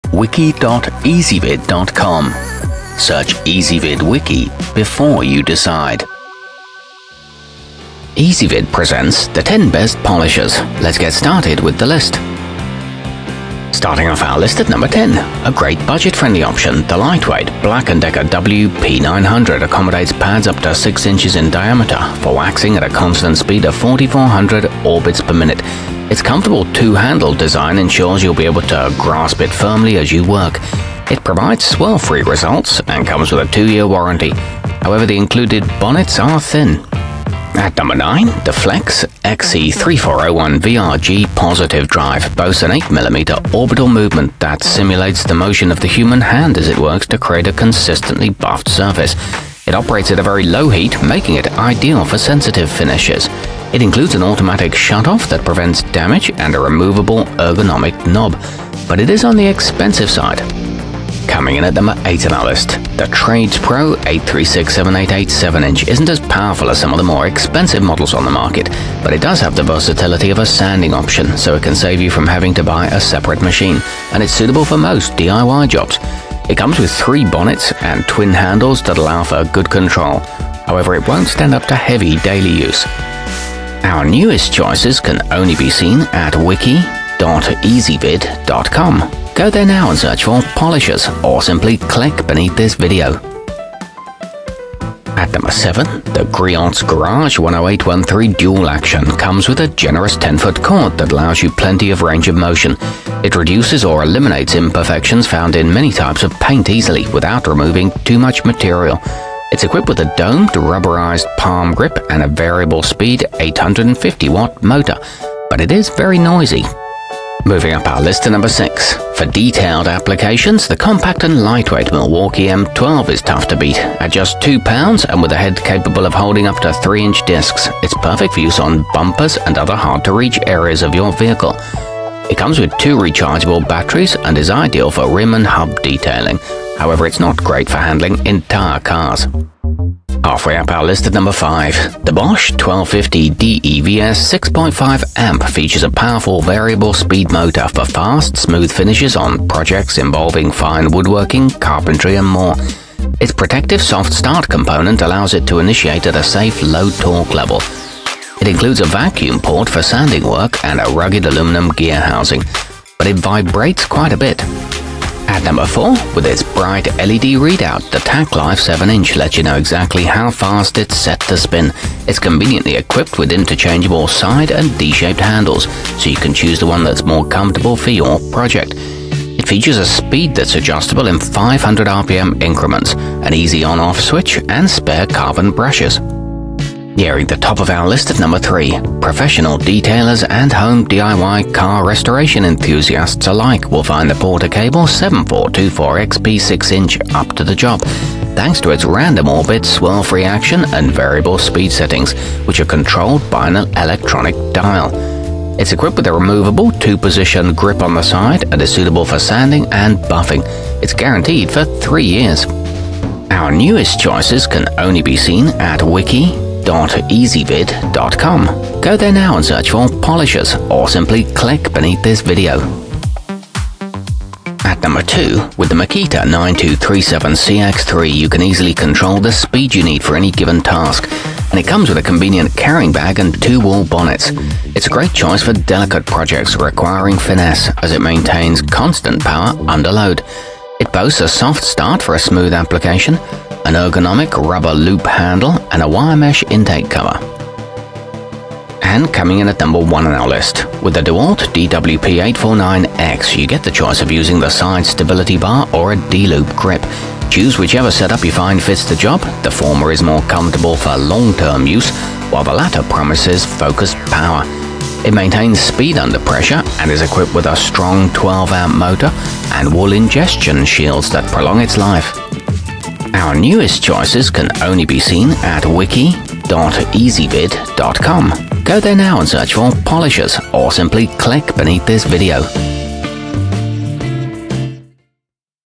10 Best Polishers 2019
Our complete review, including our selection for the year's best polisher, is exclusively available on Ezvid Wiki.

Polishers included in this wiki include the makita 9237cx3, porter-cable 7424xp 6-inch, milwaukee m12, bosch 1250devs 6.5 amp, black+decker wp900, tacklife 7-inch, flex xc3401vrg positive drive, griot's garage 10813 dual action, dewalt dwp849x, and tradespro 836788 7-inch.